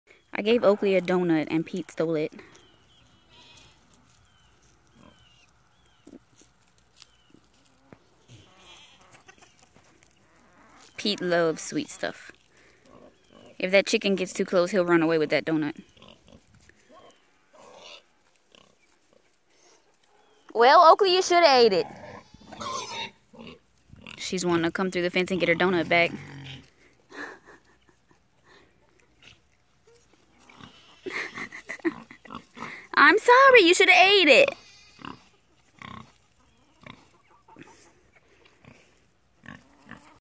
Pete stole Oakley's doughnut
Peacocks like sweet stuff